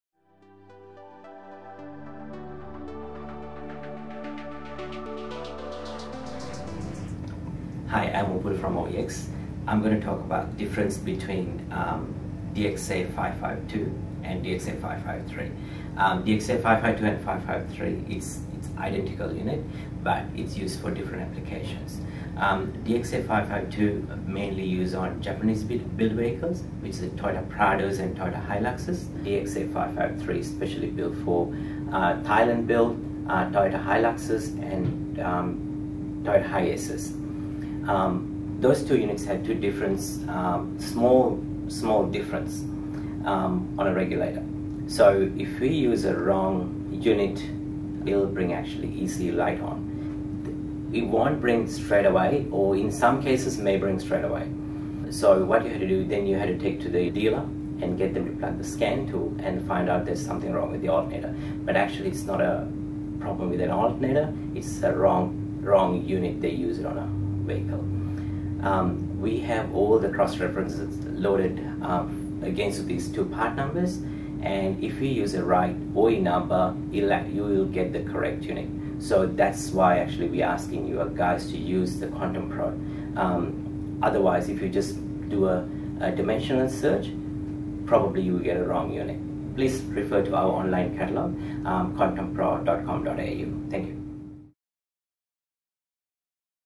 Alternators - small change, big difference
Some alternators and starter motors look the same but function very differently. A prime example are the OEX alternators, DXA552 & DXA553. Avoid installing the wrong part to your car (or in this instance, Toyota HiLux) by understanding how these products differ.